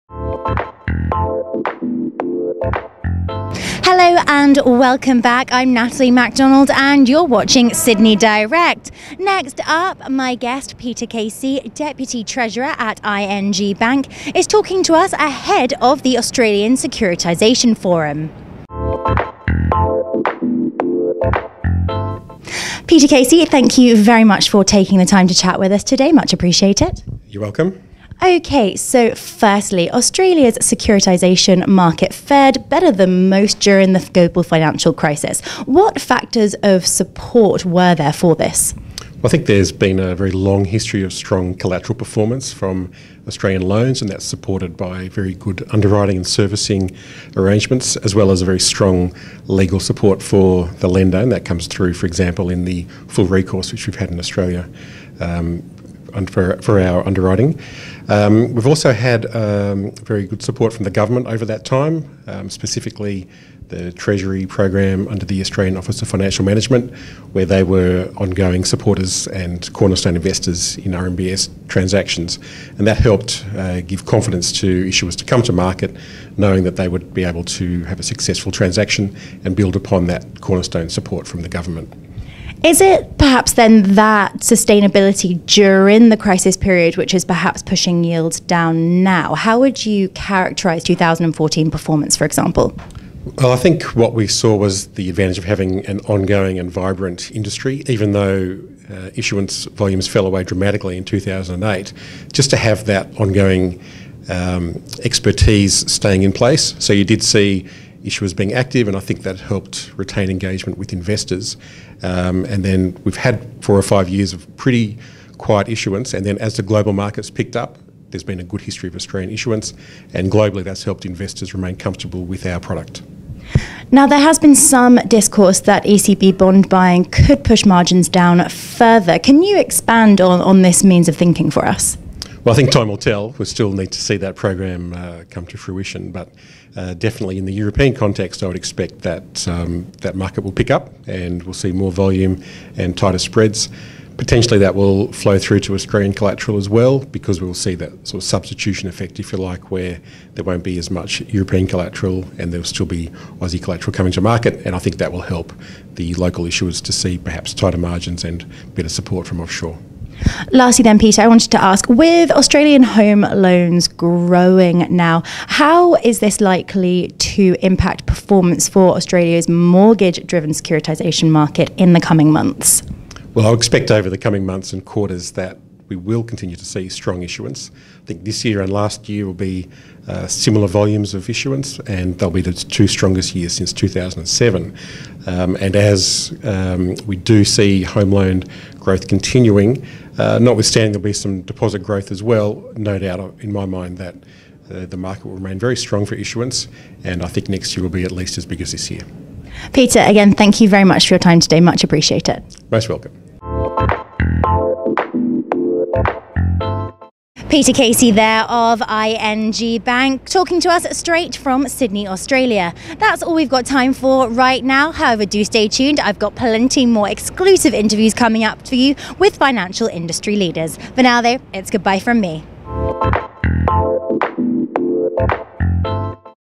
Super Securitisation
Australia’s mortgage-driven securitisation market. Peter Casey, Deputy Treasurer, ING Bank Australia.